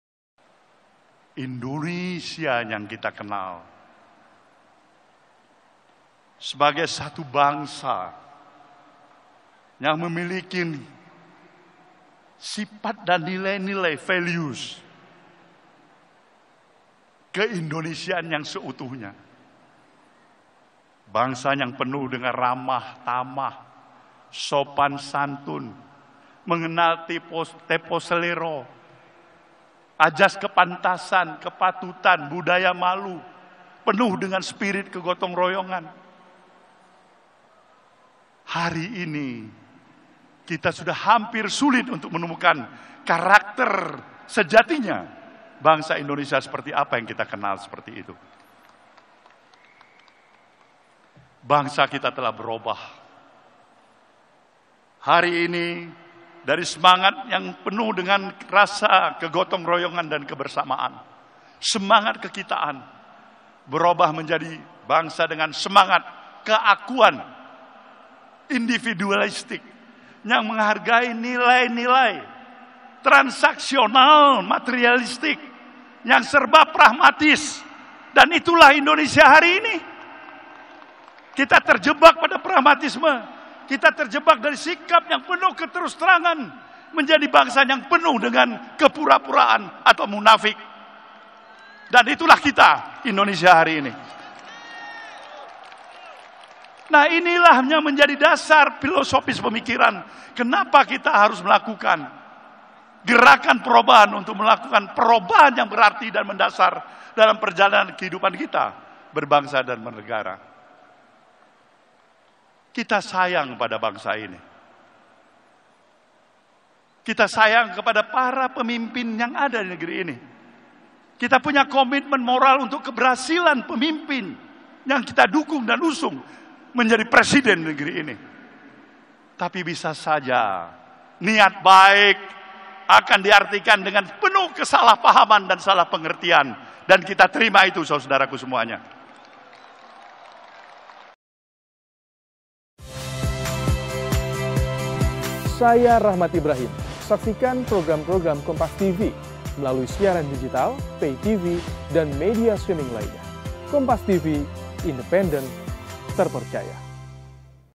Ketua Umum Partai Nasdem Surya Paloh Katakan Ini Terkait Kondisi Indonesia
JAKARTA, KOMPAS.TV - Ketua Umum Partai Nasdem Surya Paloh dalam apel siaga perubahan Partai Nasdem mengungkapkan kegundahannya terhadap bangsa Indonesia.

Menurut Surya Paloh, bangsa Indonesia saat ini telah kehilangan karakternya. Paloh mengatakan sekarang Indonesia menjadi bangsa yang materialistis dan bangsa yang penuh dengan kepura-puraan dan munafik.

Hal tersebut disampaikan Surya Paloh di Stadion GBK pada Minggu (16/7/2023). (*)

Kompas TV
Independen | Terpercaya